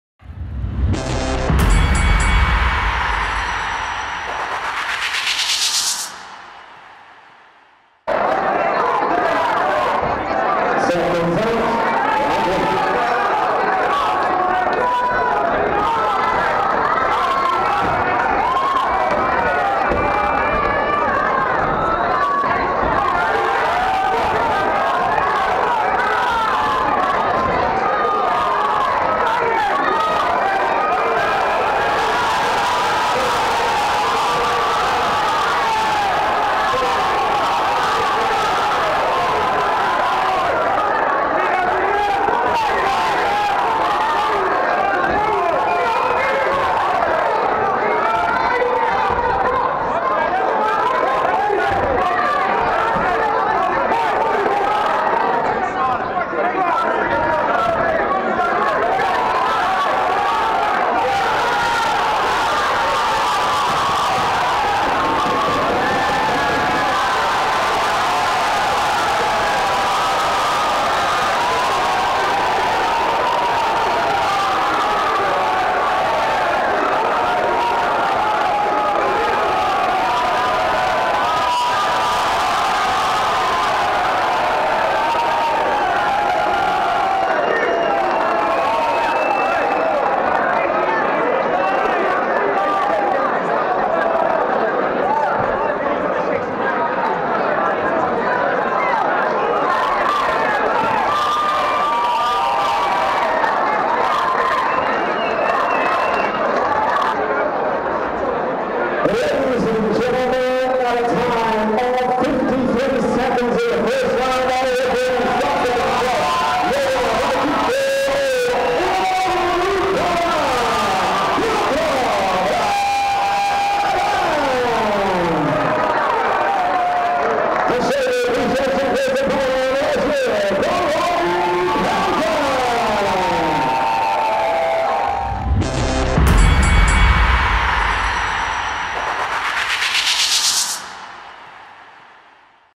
PowerBox Promotions | BATTLEGROUND | Piotr Urban Vs Robbie Payton
Robbie Payton (Red Corner) Vs Piotr Urban (Blue Corner)
BATTLEGROUND at Liquid Uxbridge 9th December 2017